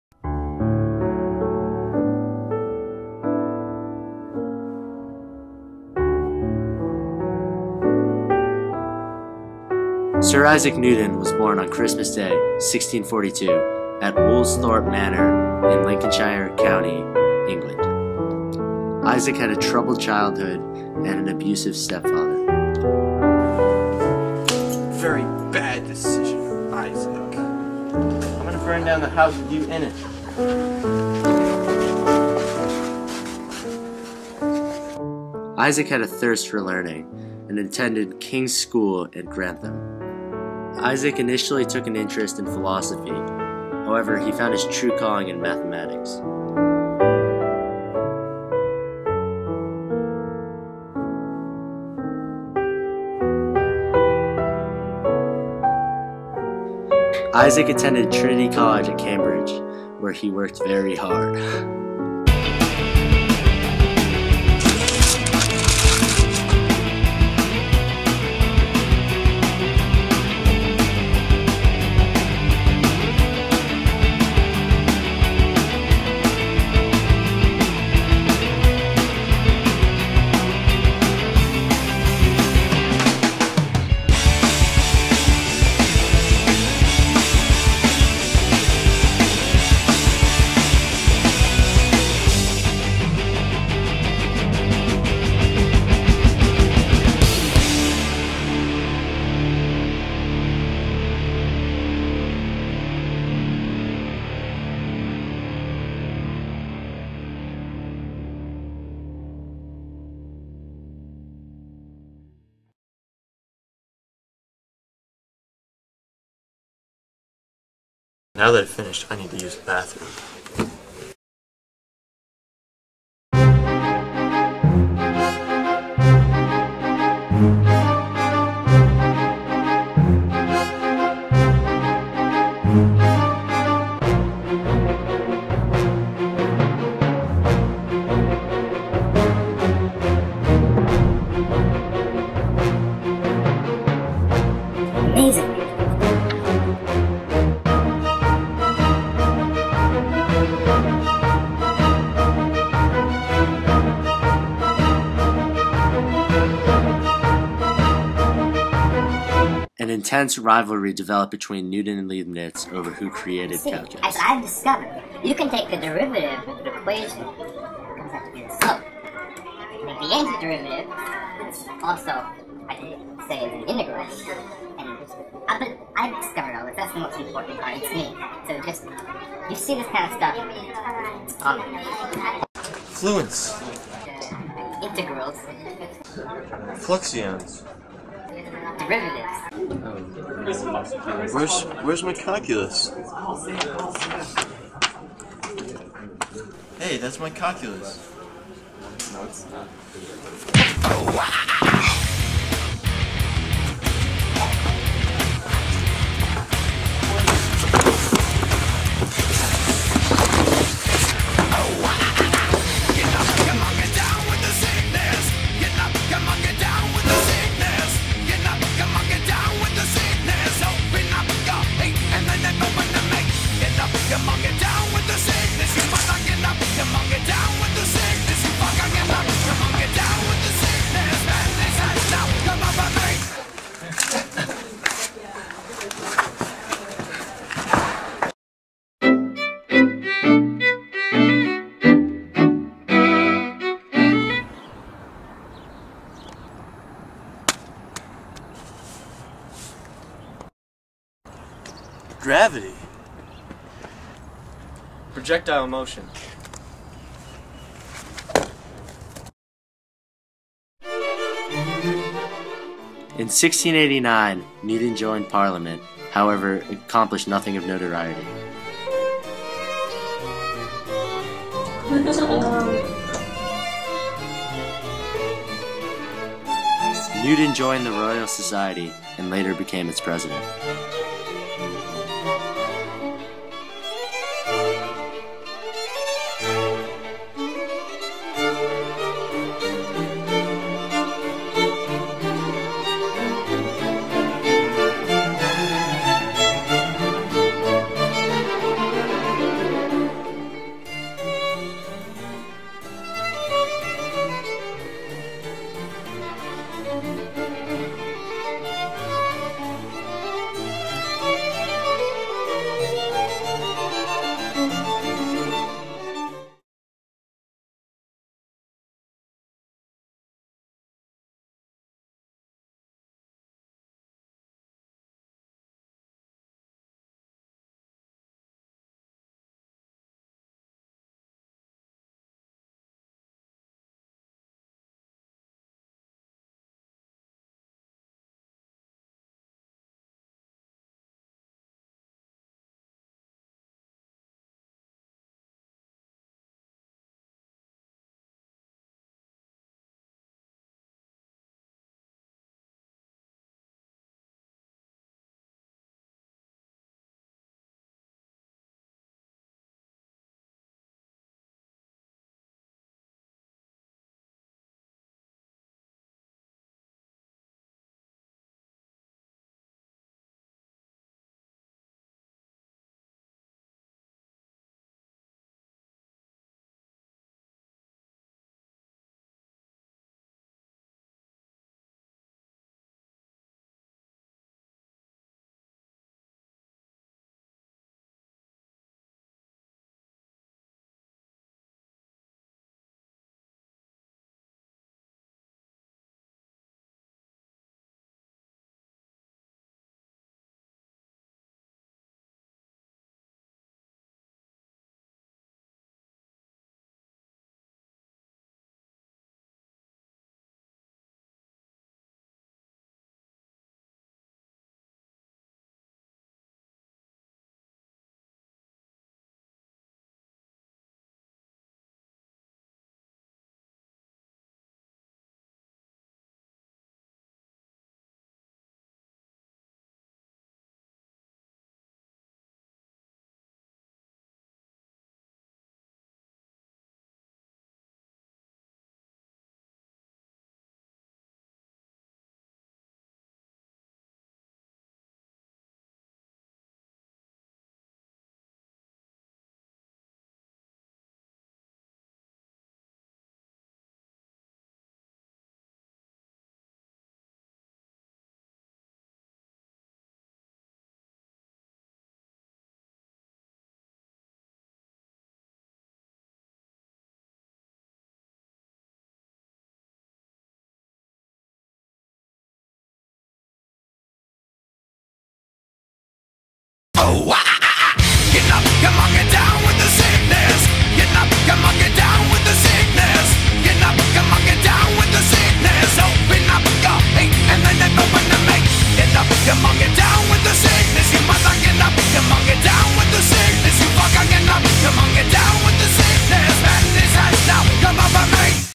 Isaac Newton: The Man, the Math, the Legend
An in-depth look at the life of Isaac from birth to death. AP BC Calc final Project.